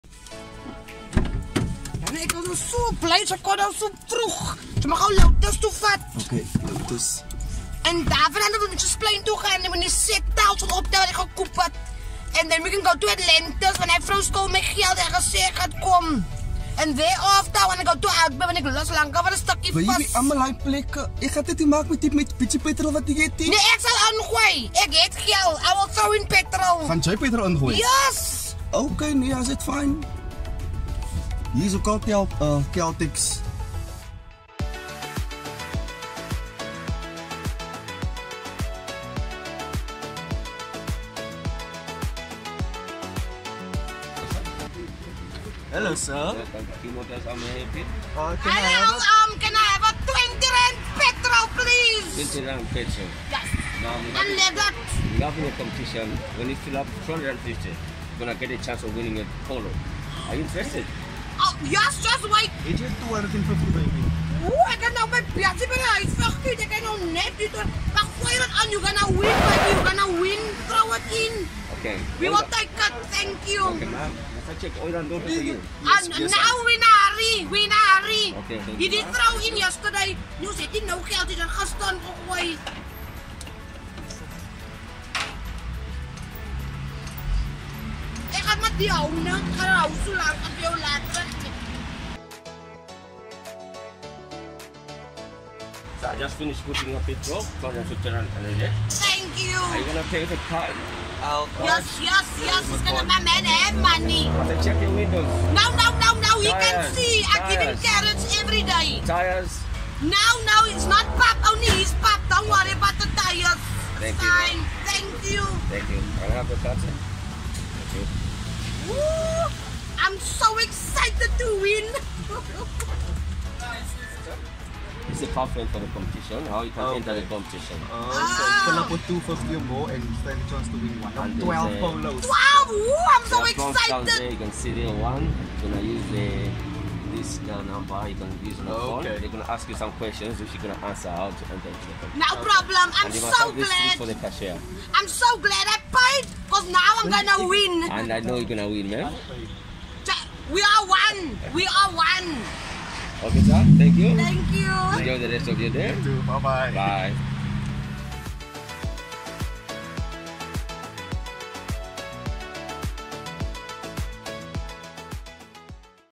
Caltex Grassy Park 🚙 ⛽
Mo is ready to win! 💥💃

Get down to CALTEX GARAGE GRASSY PARK and stand a chance to win when you fill up with R250 or more! ⛽

Lucky prizes up for grabs are:

- 1 of 12 VW POLO NF 1.0 TSI TRENDLINES

Or

- R500 FUEL VOUCHERS DAILY!